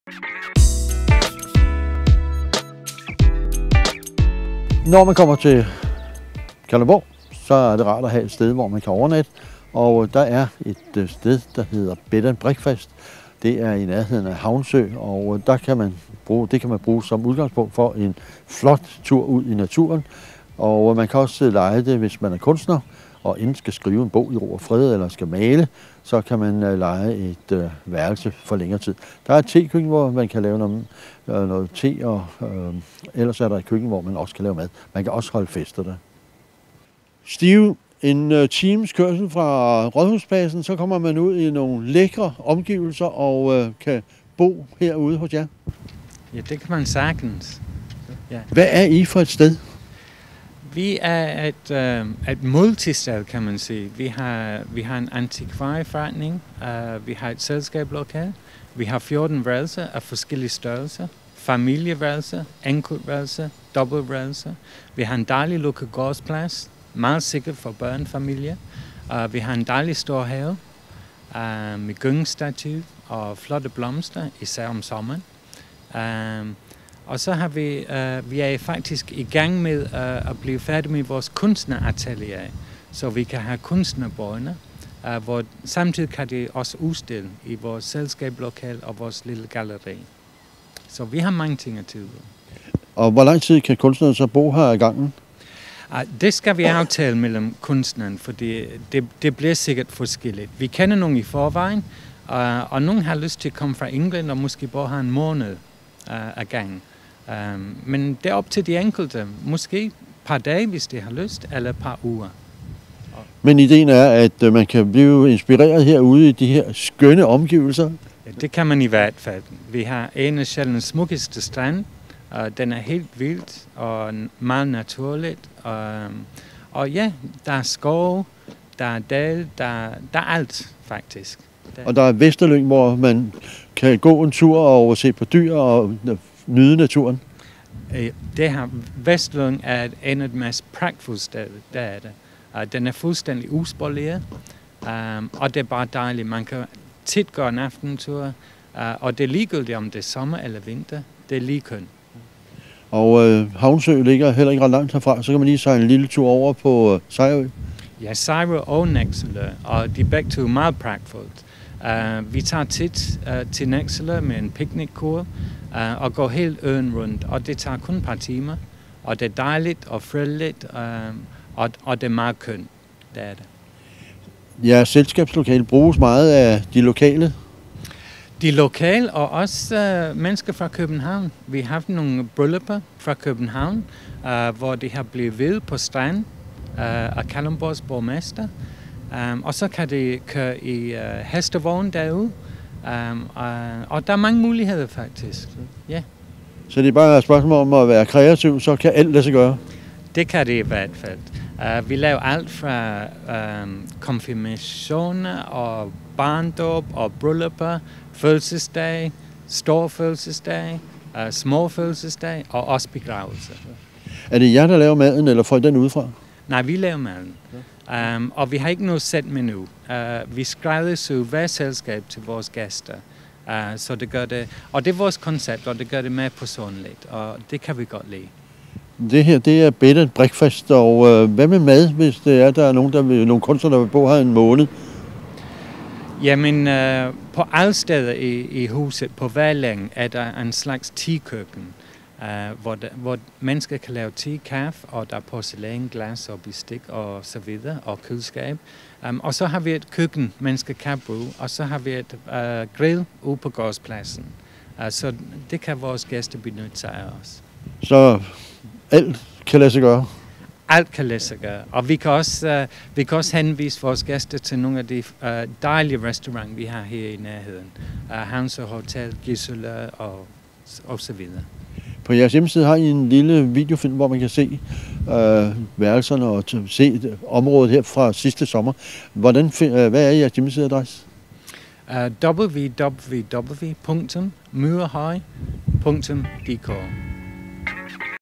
Myrehøj Bed & Breakfast
Steve Rutherforth fortæller om Myrehøj.